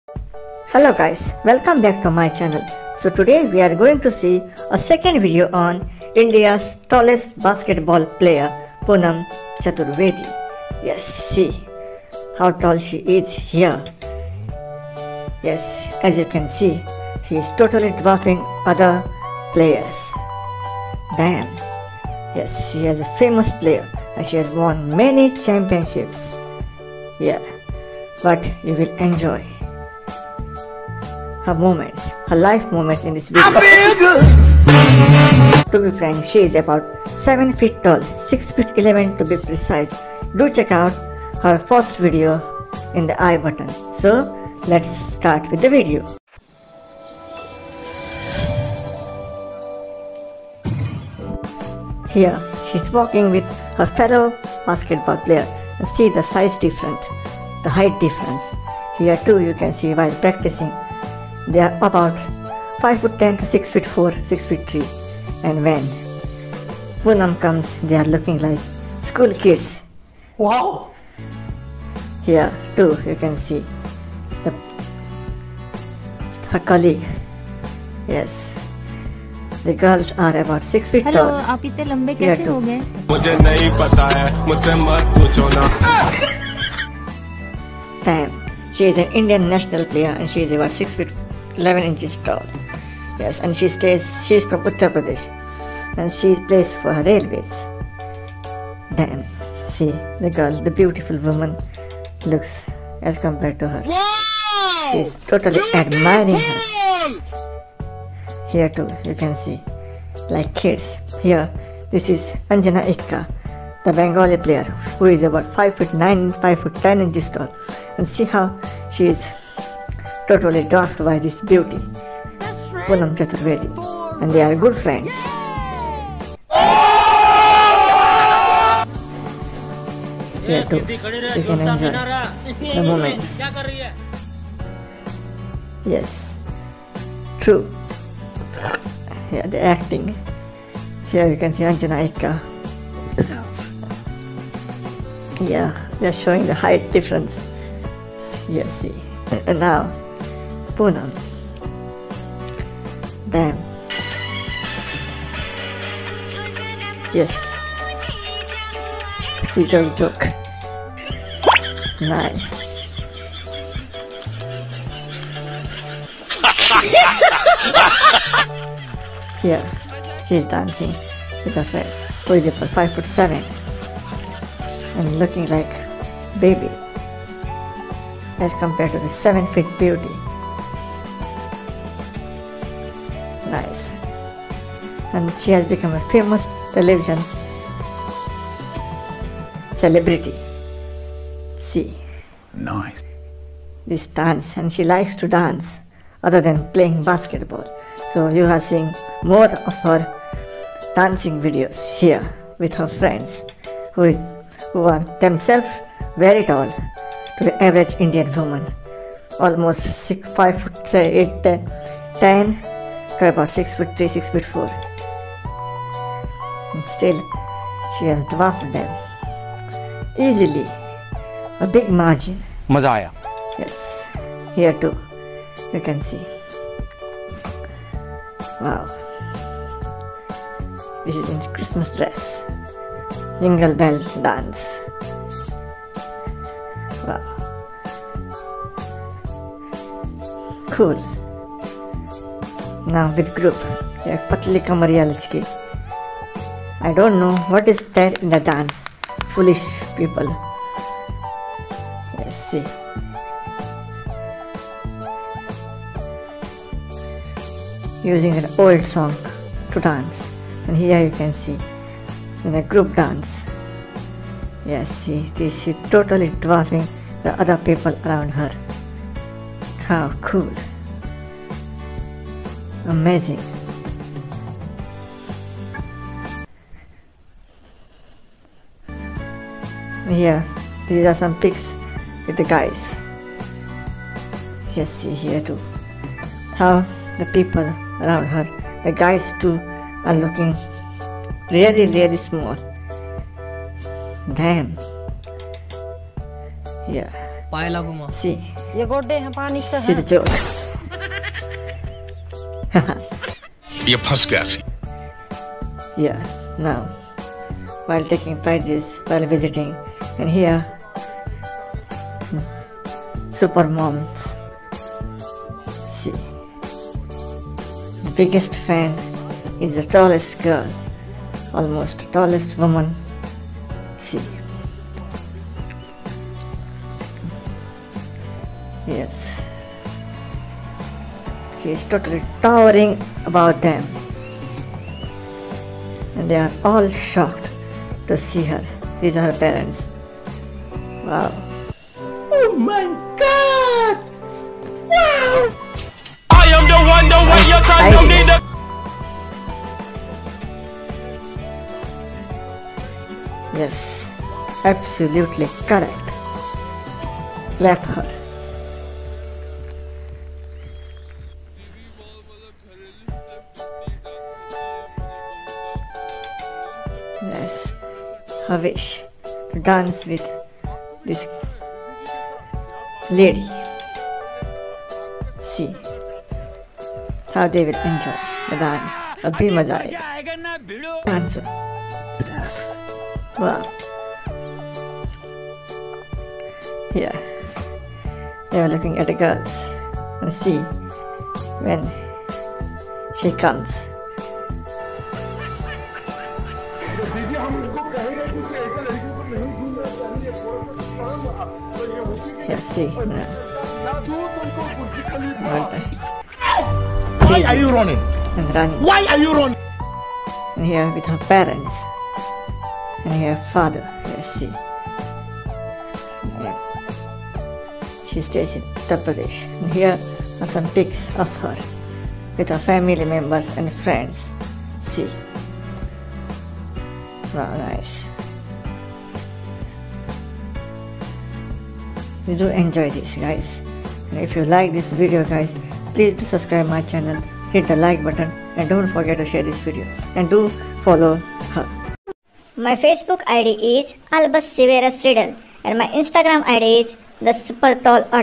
Poonam Chaturvedi - India's tallest woman basketball player | tall indian woman | tall indian girl
Jai Siyaram

This is the 2!nd video on India's Tallest woman basketball player Poonam Chaturvedi.  Do Enjoy it guys.